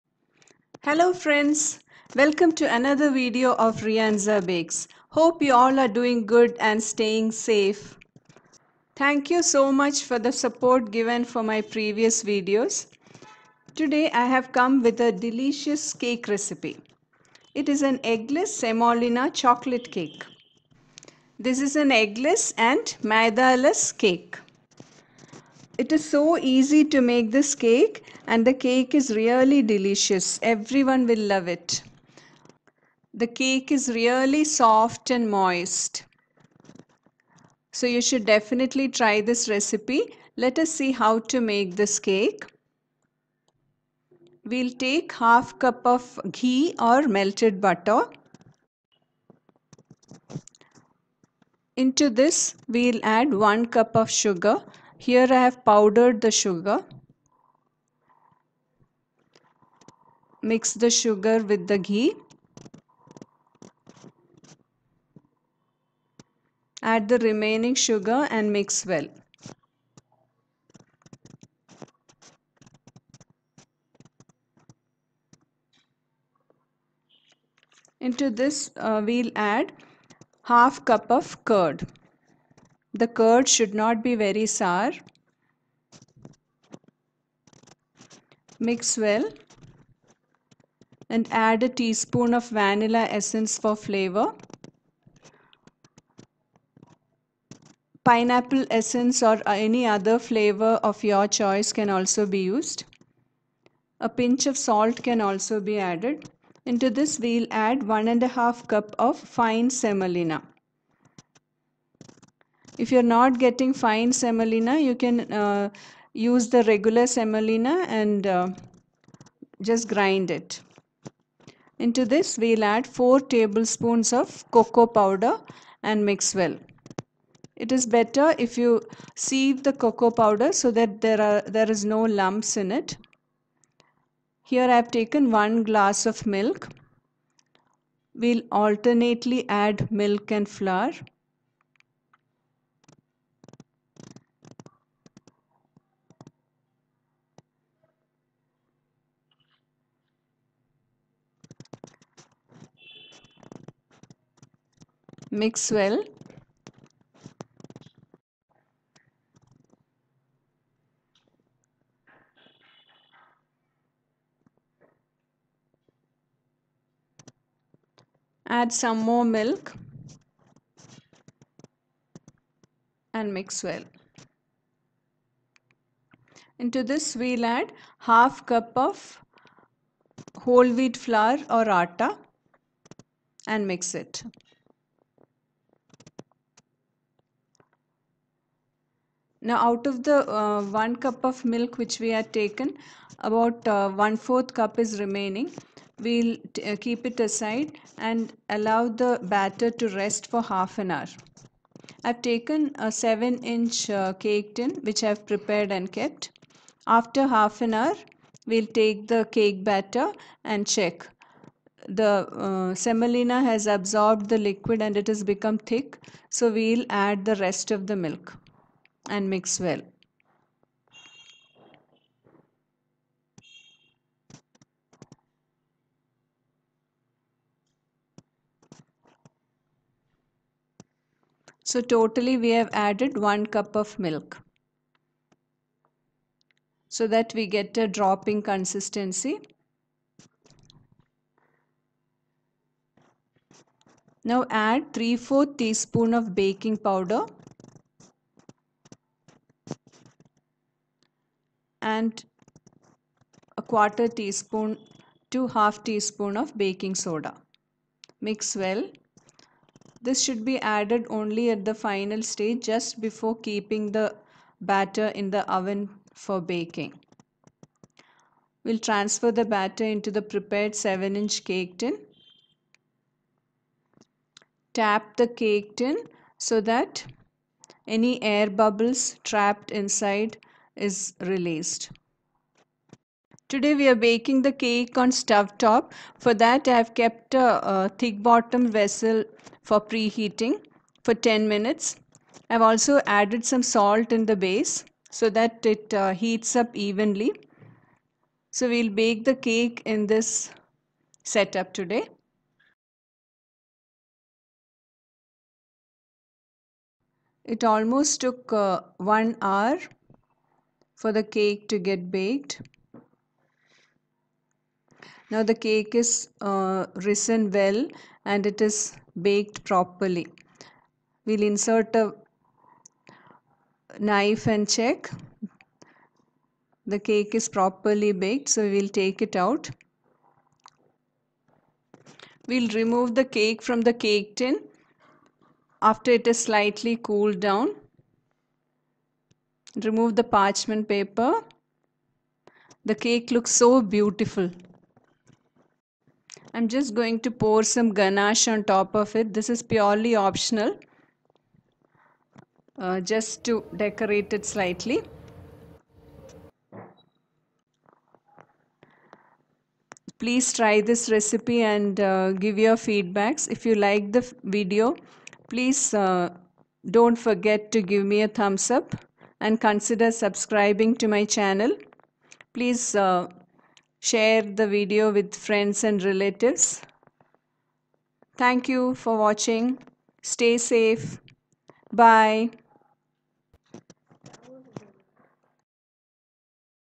Suji Chocolate Cake| Eggless Chocolate Cake without Oven | Eggless Rava Chocolate Cake| रवा केक # 37
In this video we will see how to bake an eggless semolina chocolate cake made using semolina and wheat flour or atta without oven.
Measuring cup used 200 ml.
This cake can be baked at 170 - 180 degree celsius  to 1 hour in a preheated oven. It might vary slightly depending on the oven and size of the cake tin used.

Ingredients
Melted butter/Ghee - 1/2 cup (100 gm)
Sugar - 1 cup (200 gm)
Curd -1/2 cup
Semolina - 11/2 cup (215 gm)
Wheat flour - 1/2 cup (55 gm)
Salt..a pinch
Milk - 1cup (200ml)